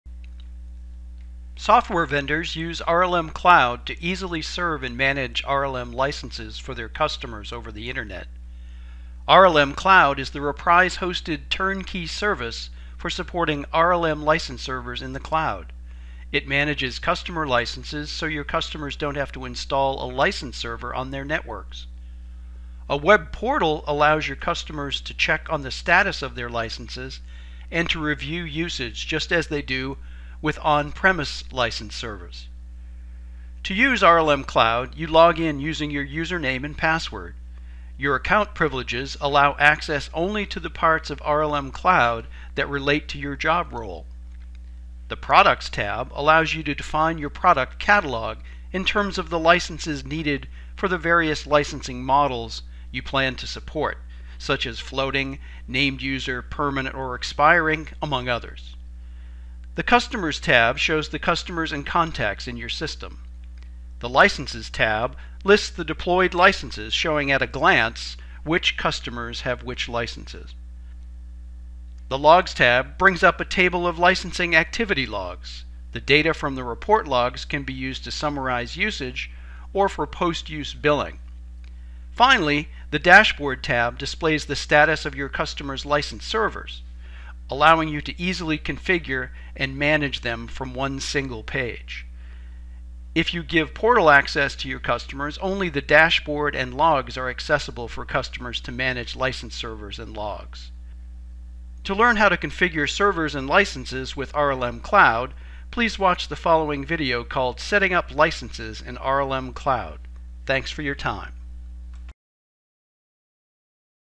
Intro to RLM Cloud
Software vendors use RLM Cloud to easily serve and manage RLM licenses for their customers over the Internet.

RLMCloud is the Reprise-hosted turnkey service for supporting RLM license servers in the Cloud. It manages customer licenses so your customers don't have to install a license server on their networks.

A web portal allows your customers to check on the status of their
licenses, and to review usage, just as they do with an on-premises
license server.

To use RLM Cloud, you log in using your username and password.

Your account privileges allow access only to the parts RLM Cloud that relate to your job role.

The "Products" Tab allows you to define your product catalog in terms of the licenses needed for the various licensing models you plan to support, such as: floating, named-user, permanent or expiring, among others.

The "Customers" tab shows the customers and contacts in your system.

The "Licenses" tab lists the deployed licenses, showing, at a glance,
which customers have which licenses.

The "Logs" tab brings up a table of licensing activity logs. The data
from the Report Logs can be used to summarize usage or for post use billing.

Finally, the "Dashboard" tab displays the status of your customer's
license servers, allowing you to easily configure and manage them from a single page.

If you give portal access to your customers, only the "Dashboard" and "Logs" are accessible for customers to manage license servers and logs.

To learn how to configure servers and licenses, please watch the
following video called "Setting up Licenses in RLM Cloud."